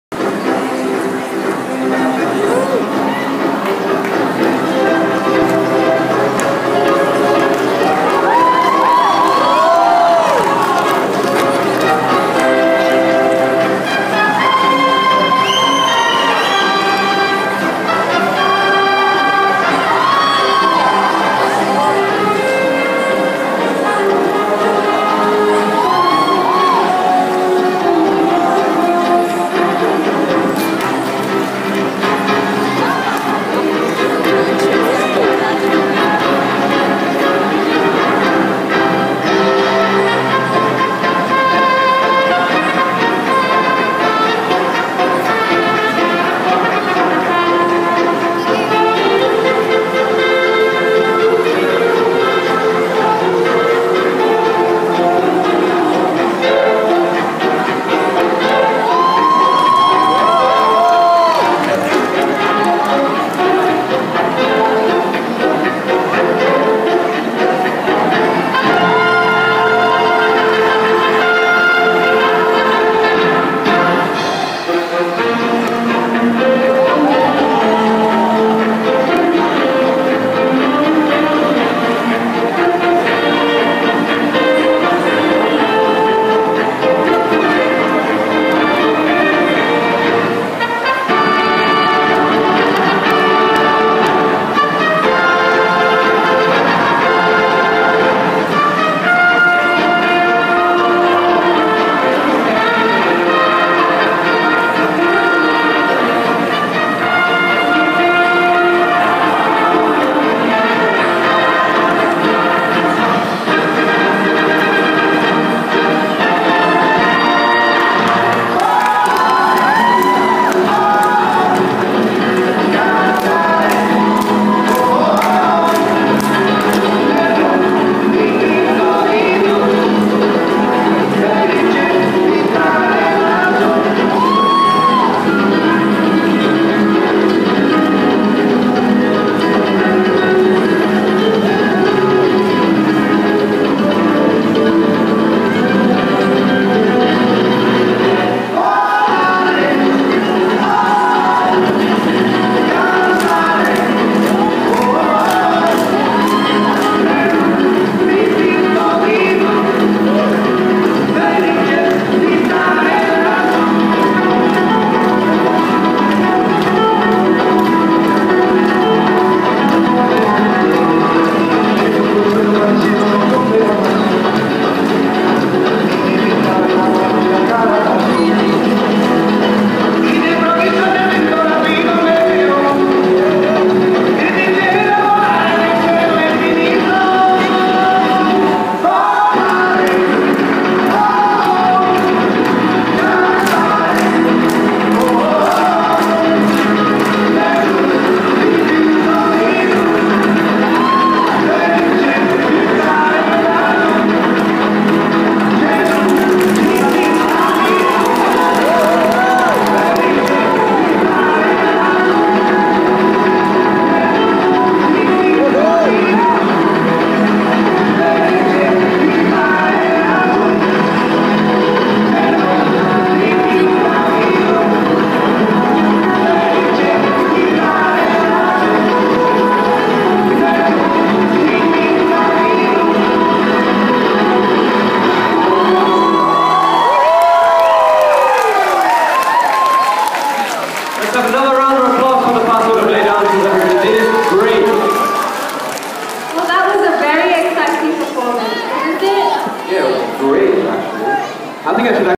Spanish Dance performed by Year 5
Performance by Year 5 during International Day celebrations held on 18 Oct 2013